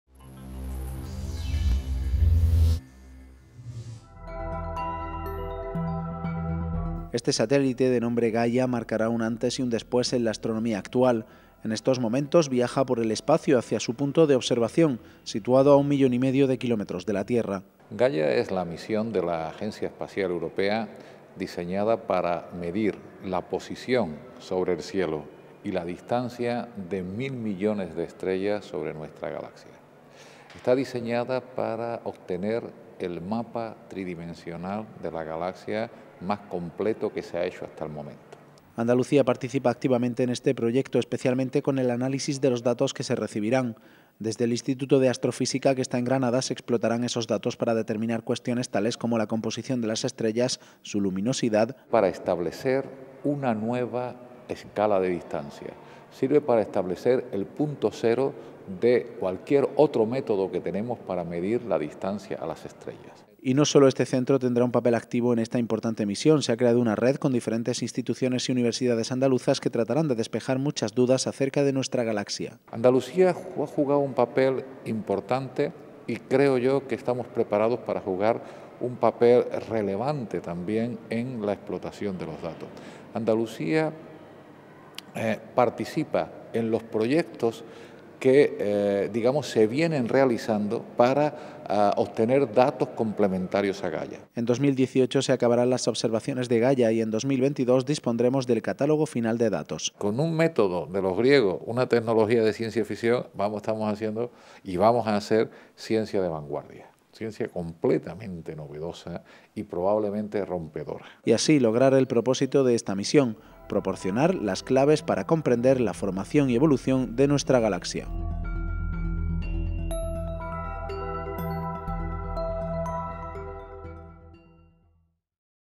HDL Científicos andaluces analizarán datos del satélite Gaia para hacer el mapa 3D de la galaxia
El Instituto de Astrofísica de Andalucía es uno de los organismos que participa en el proyecto Gaia, un satélite de la Agencia Espacial Europea que se acaba de lanzar al espacio con el objetivo de analizar las estrellas y conseguir la elaboración de un mapa en tres dimensiones de la galaxia. El organismo granadino es uno de los centros donde se analizarán los datos que el satélite vaya enviando a la Tierra. Emilio Alfaro nos habla de la implicación andaluza en este proyecto, que supondrá un antes y un después para la astronomía.